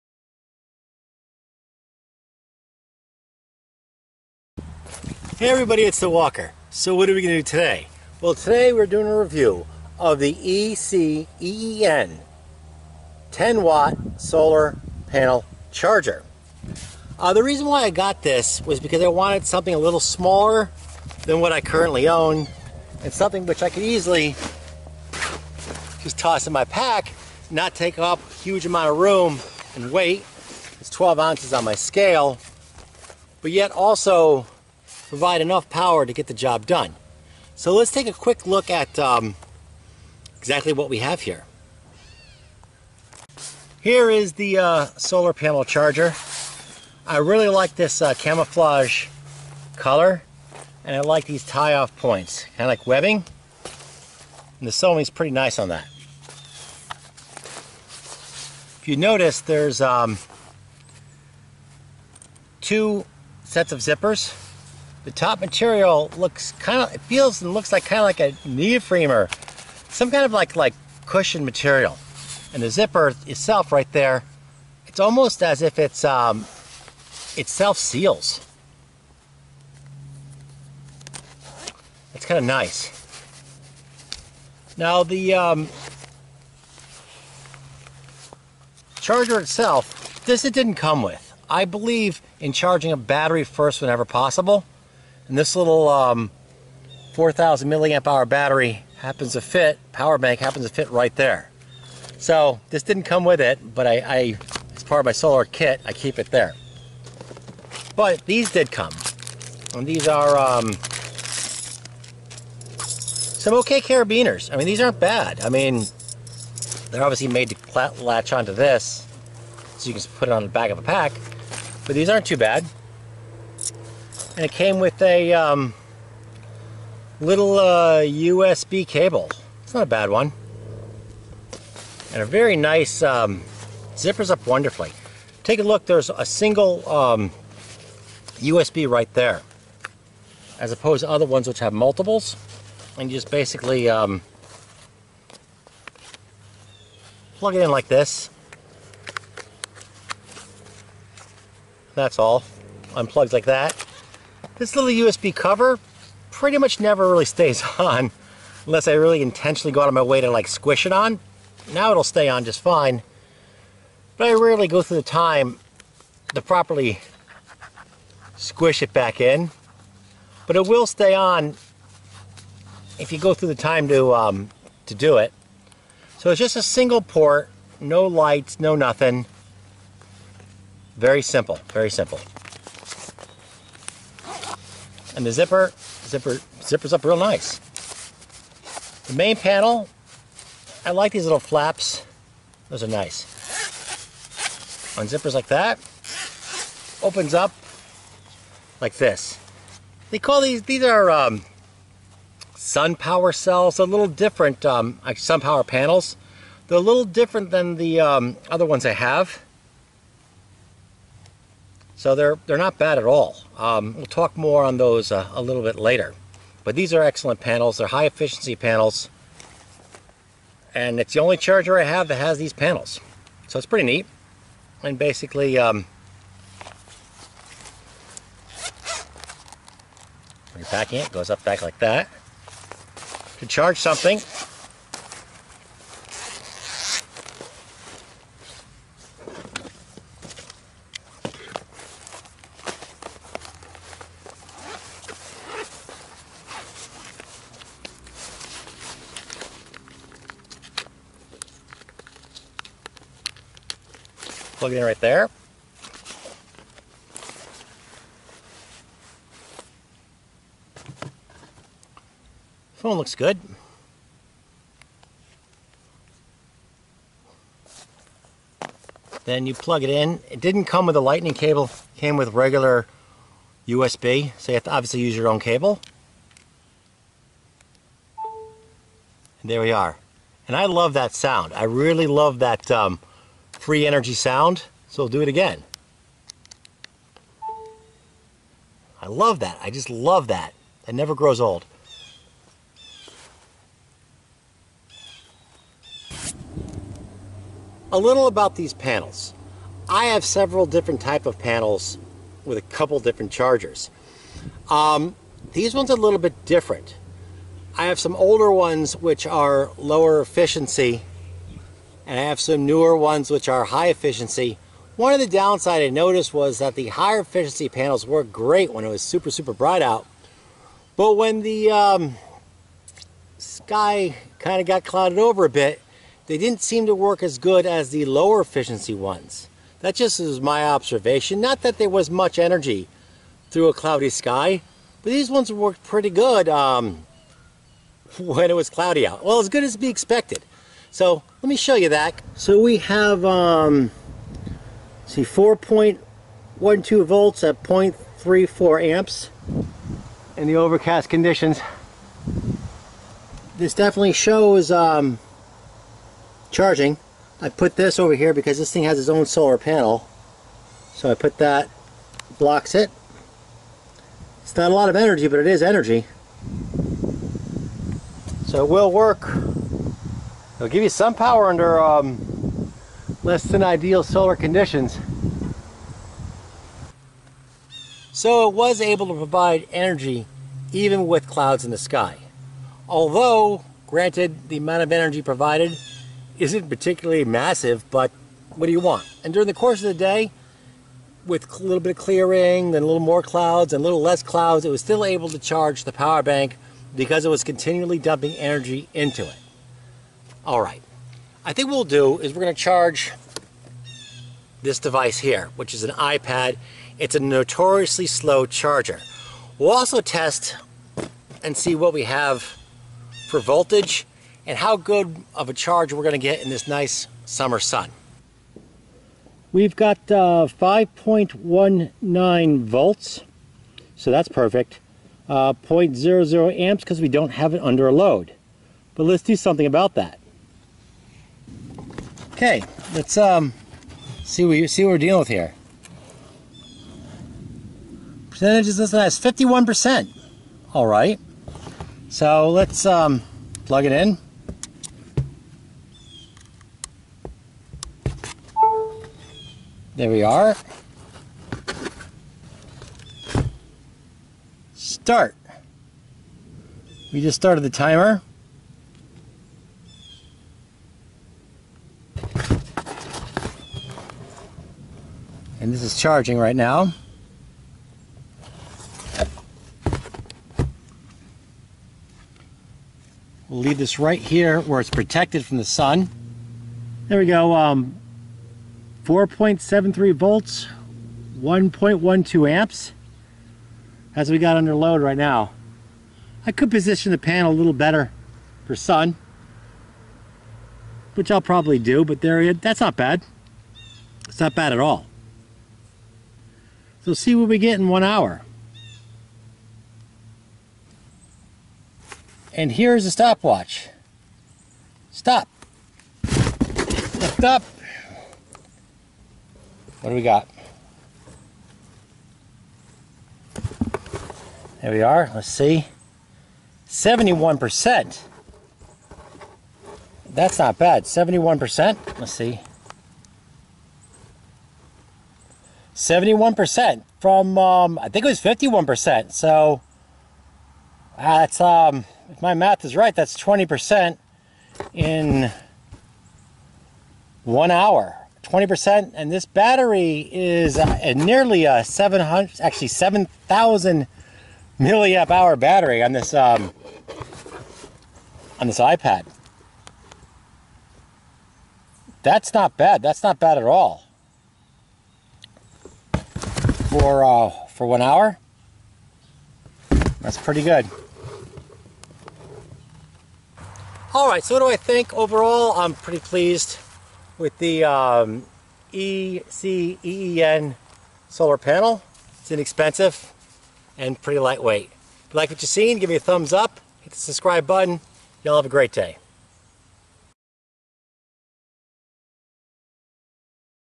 ECEEN Folding 10 Watt Solar Panel Phone Charger With USB Port Review.
ECEEN Folding Solar Panel Phone Charger With USB Port Review. The title says it all. The panel can provide 1A or more charging at 5 volts. In addition it self resets every 3 minutes. This design feature allows for fast charging as it self corrects common charging errors such as the infamous "This accessory may  not be supported" error message and termination of charge. The ECEEN is the only panel I tested which self corrects this error which is the fault of an update in IOS 10.